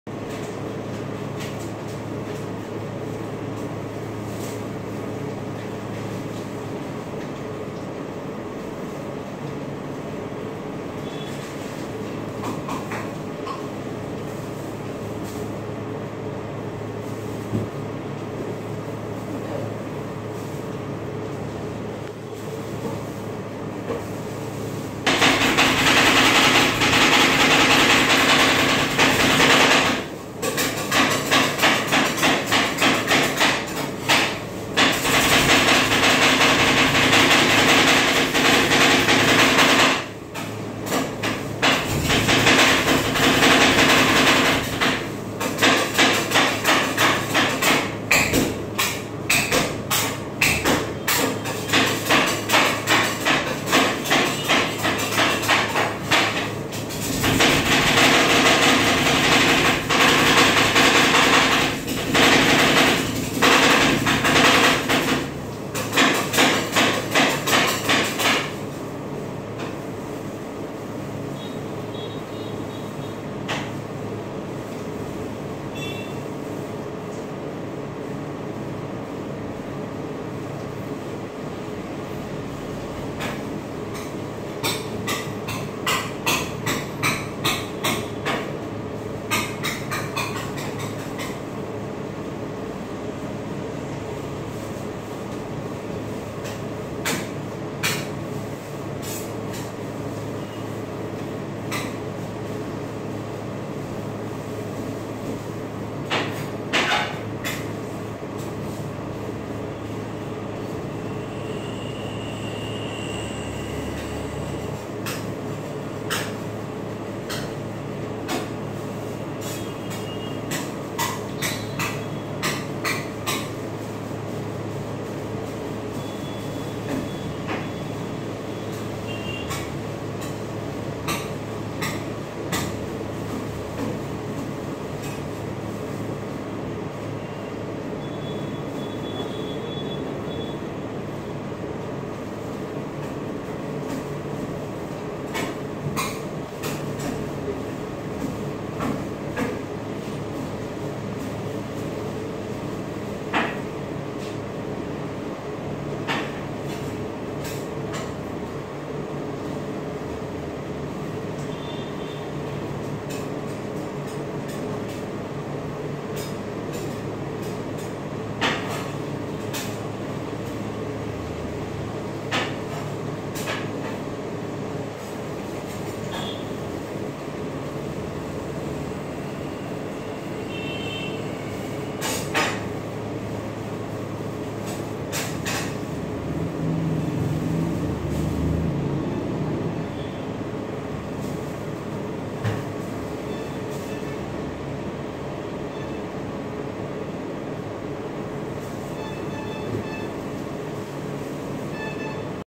Marshmallows Ice Cream ₹80 Satisfying Experiment
Marshmallows ice cream
satisfying Experiment
1) take thick vanilla ice cream Milk 100ml...
2) Marshmallows pics...
3) Strawberry Syrups 10ml...
4) Blueberry Syrups 10ml...
5) Mix it together...
6) pour into the ice cream roll machine.
Toping...
1) Strawberry Syrups...
2) Blueberry Syrups.